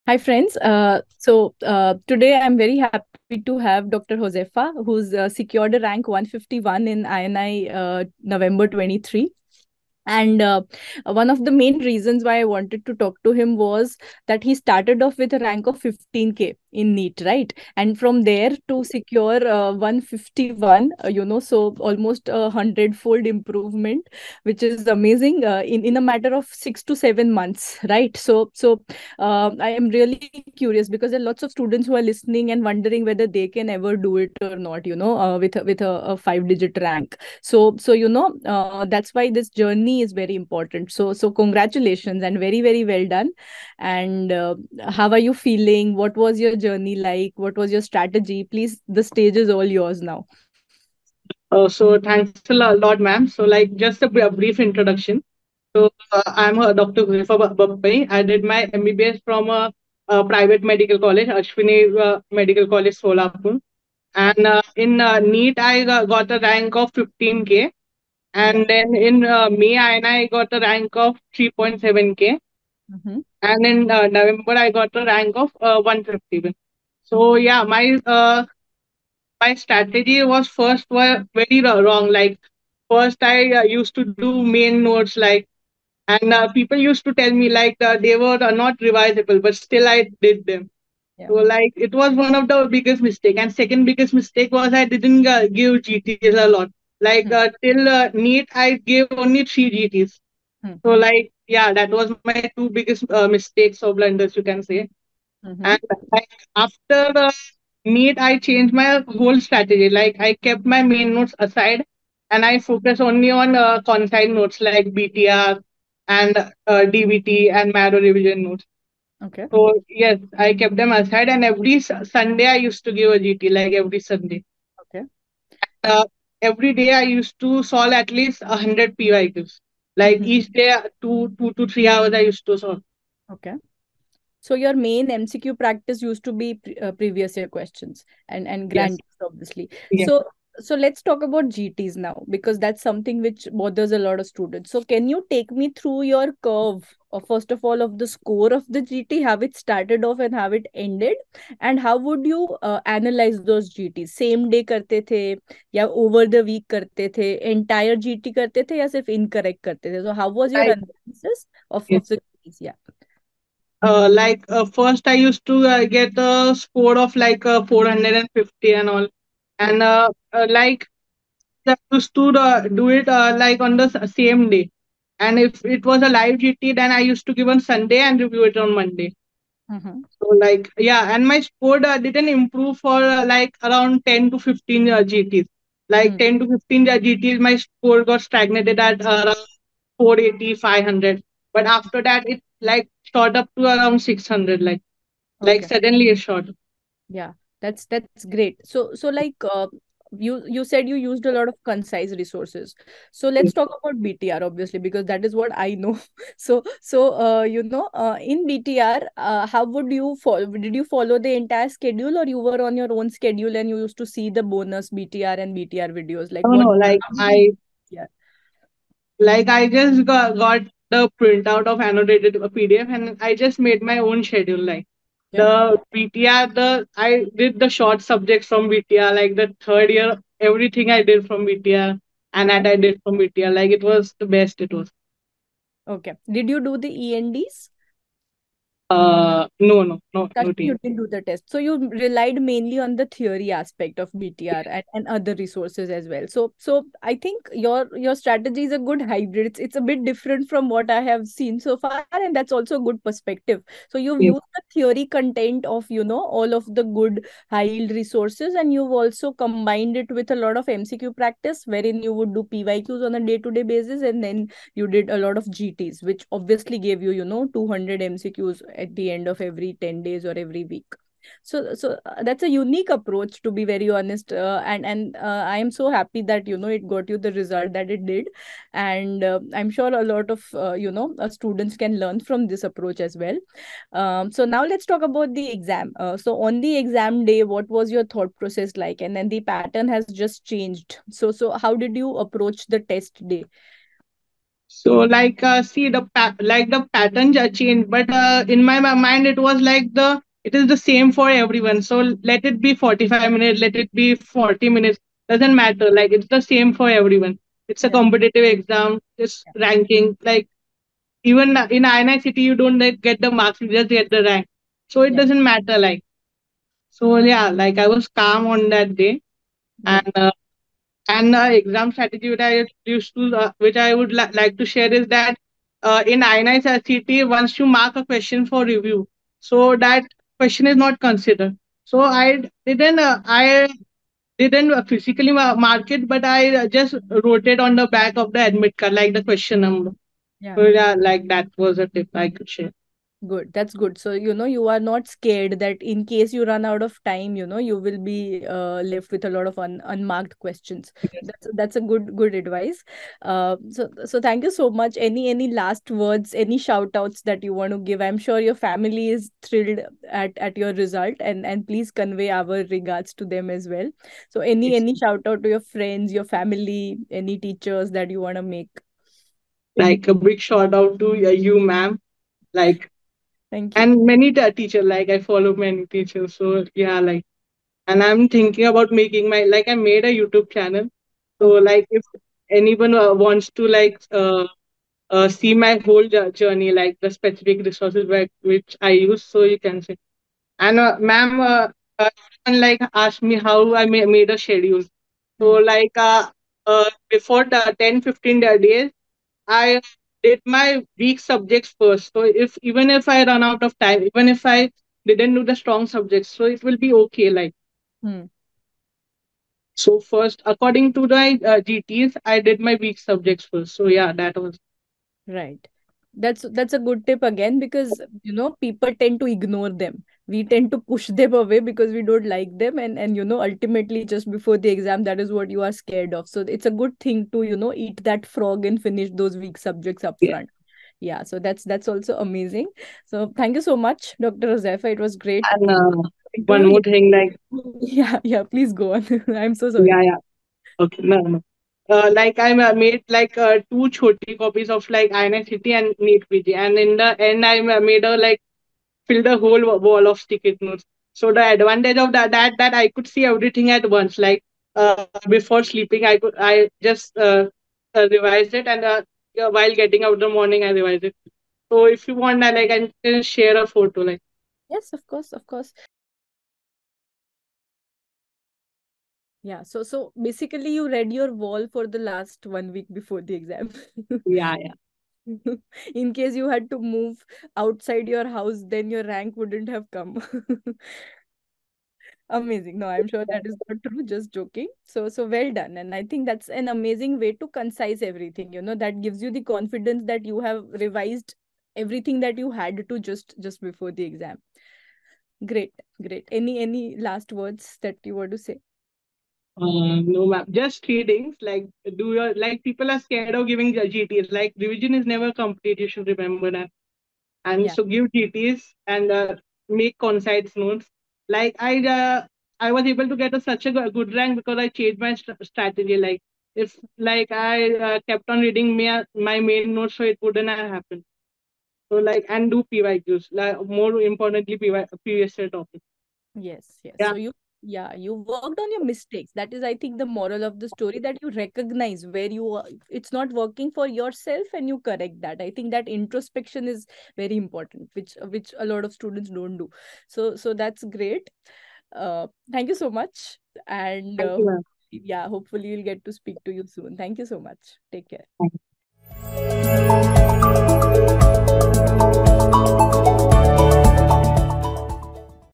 Concise notes & strategic planning: Catalysts behind Dr Huzaifa Bapai's remarkable INICET Rank- 151
Looking to ace your medical entrance exam? Join Dr. Huzaifa Bapai as he reveals the secrets to his incredible 151 rank in INI CET. Learn how concise notes and strategic planning played a key role in his success. With his help, you can prepare effectively and achieve outstanding results. Don't miss out on this educational and motivational video! Watch now for valuable insights and get ready to excel.